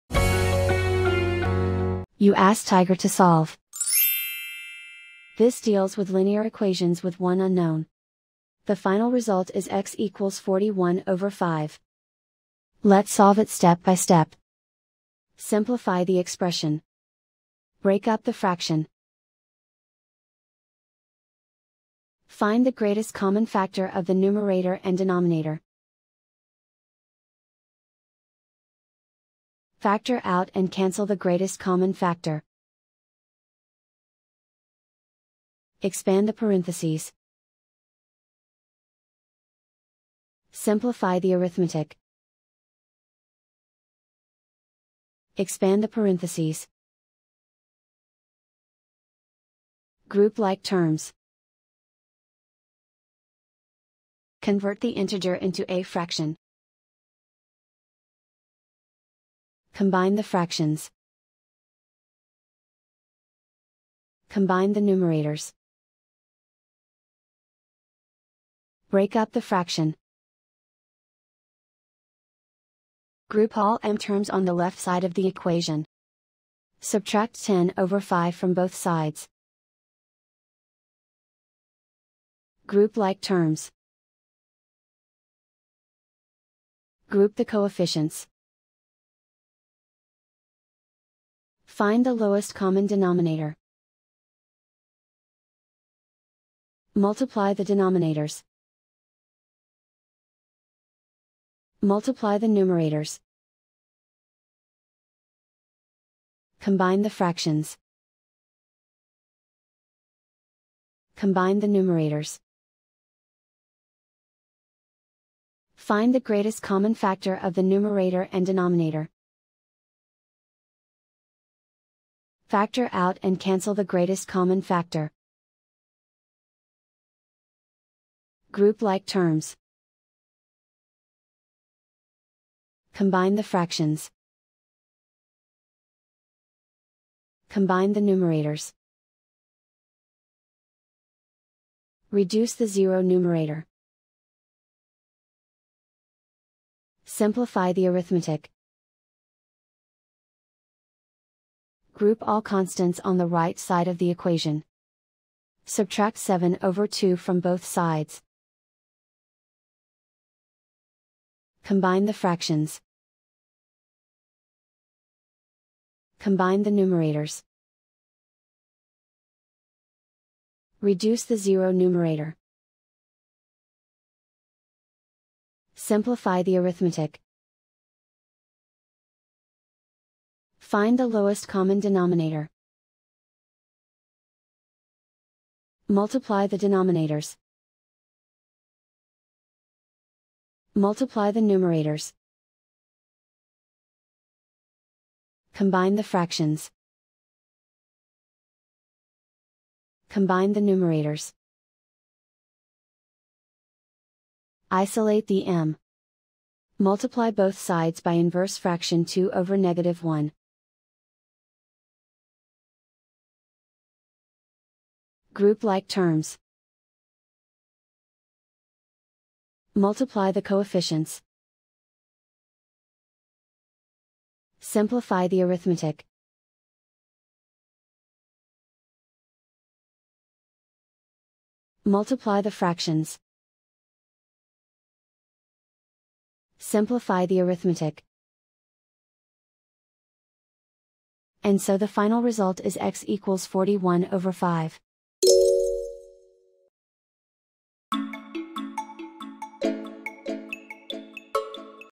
Solve 2-3(x-5)/10=(x-3)/5: Linear Equation Video Solution | Tiger Algebra
Struggling with 2-3(x-5)/10=(x-3)/5? Watch our step-by-step video on Tiger Algebra to master this linear equation effortlessly.